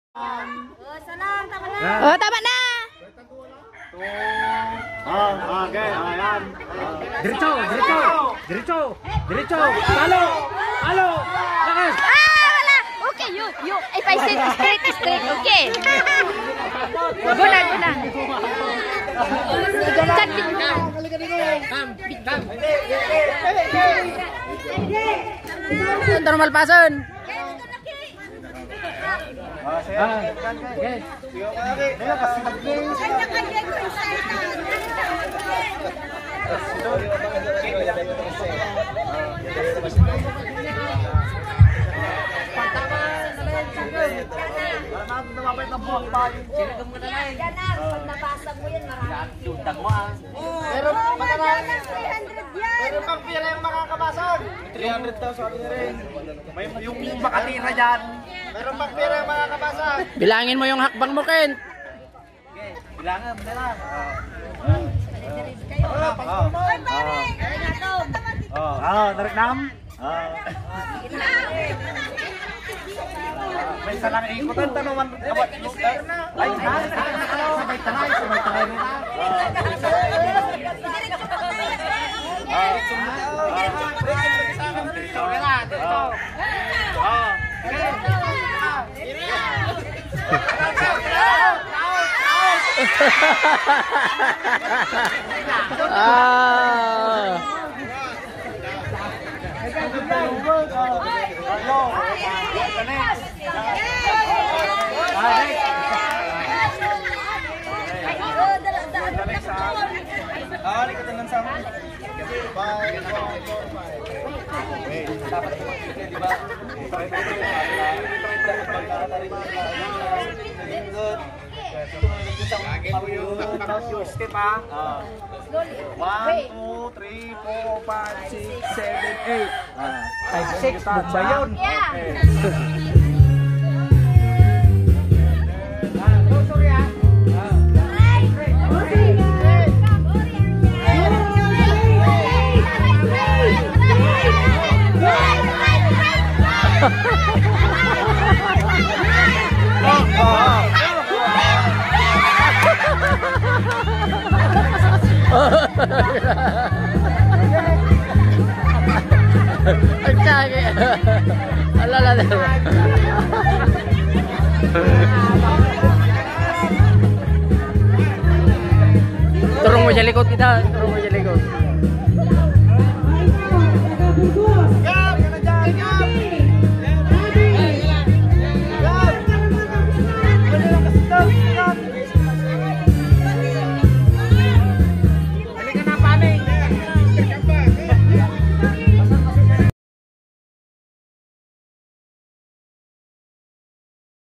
Hampas o Pukpok Palayok | Filipino Traditional Game | Larong Pinoy
A traditional game called "Hampas Palayok" (hit the pot) was very popular in the Philippines. I know that every Filipino experience playing this game at least once during their childhood. Before, this game was only played during Barrio Fiesta (Feast). This was played mostly by children but now, it can also be played by adults during birthday parties, Christmas celebrations or any other occasions.